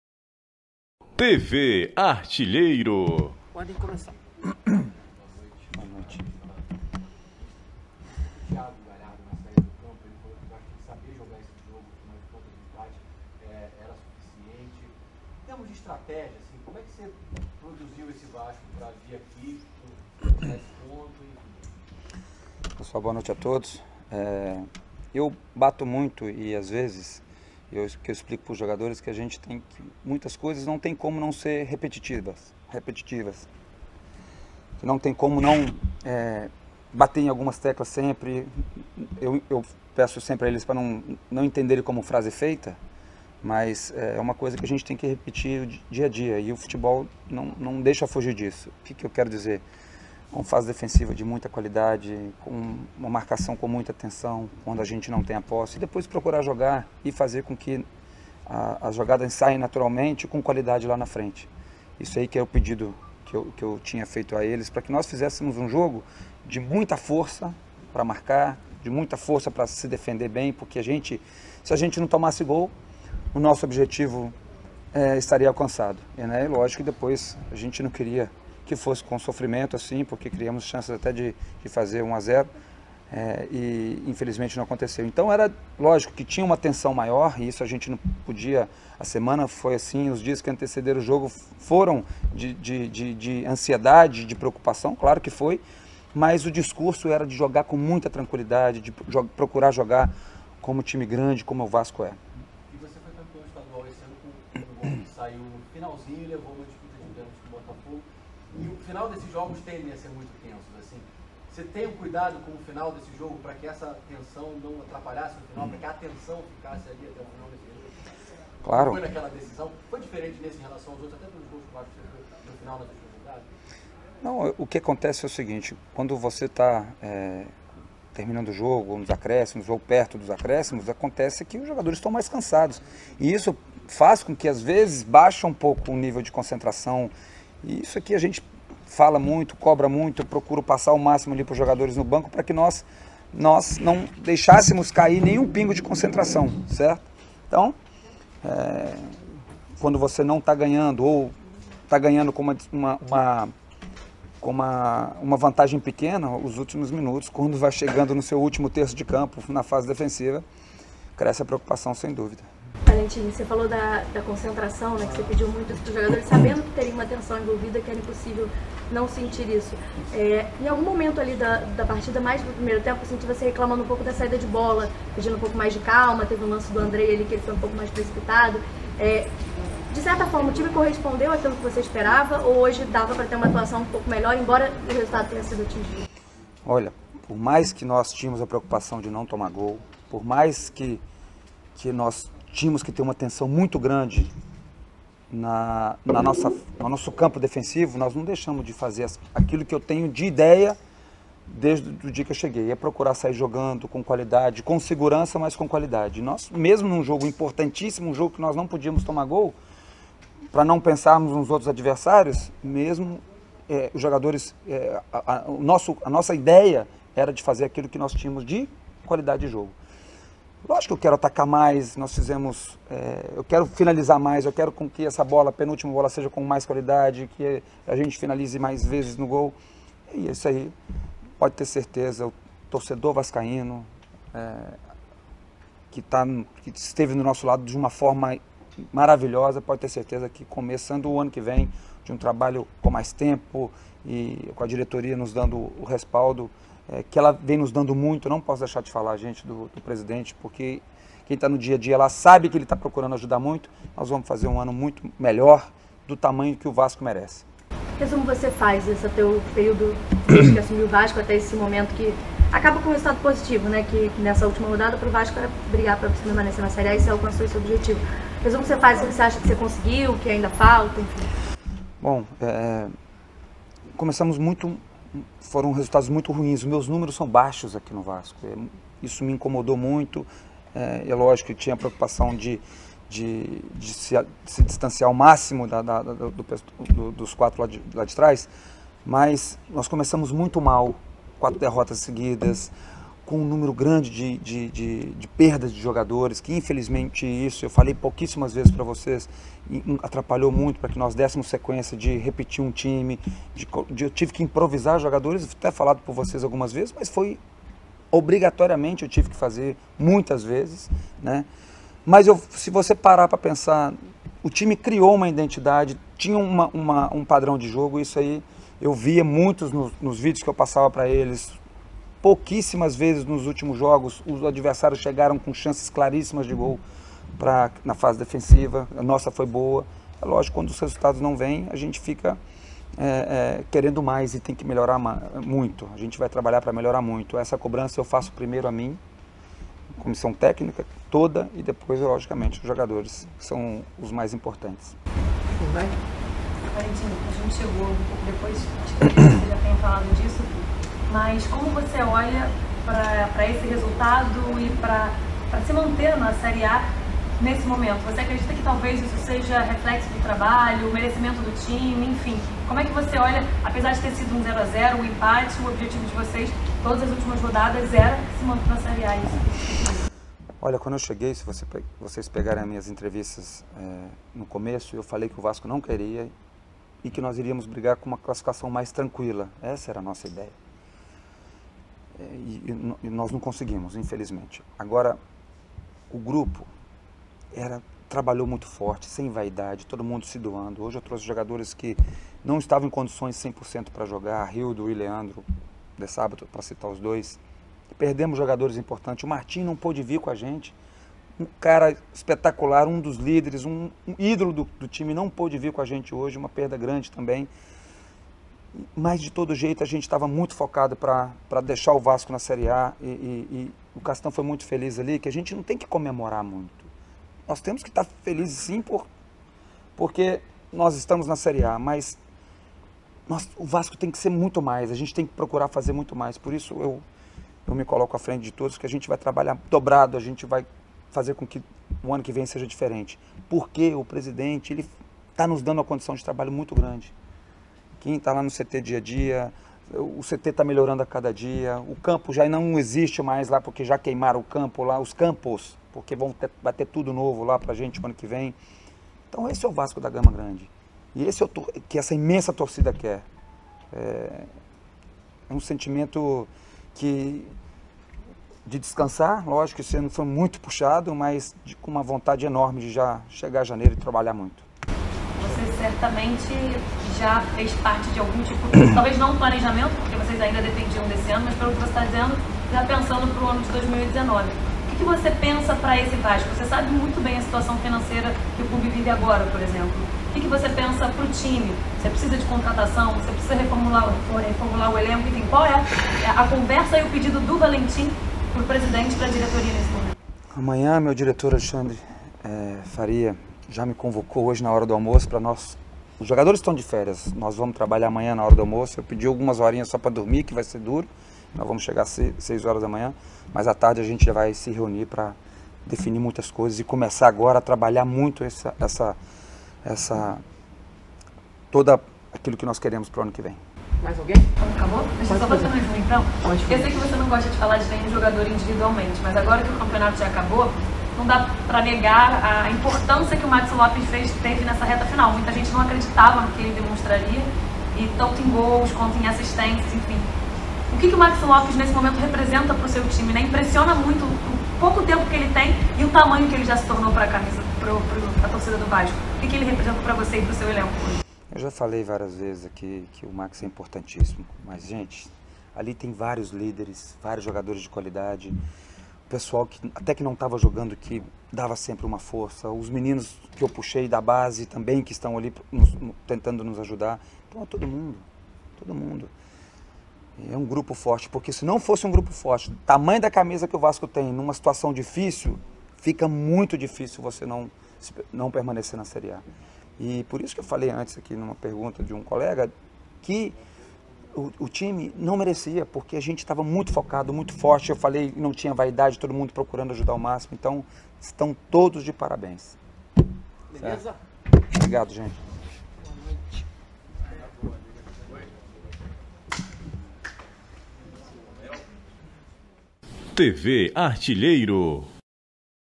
[Série A '18] Coletiva Alberto Valentim | Pós-jogo Ceará SC 0 X 0 CR Vasco da Gama | TV ARTILHEIR⚽
[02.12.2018] Coletiva treinador Alberto Valentim (CRVG) |
Pós-jogo Ceará Sporting Club 0 X 0 Clube de Regatas Vasco da Gama/RJ |
Campeonato Brasileiro Série A '18 | 38ª Rodada |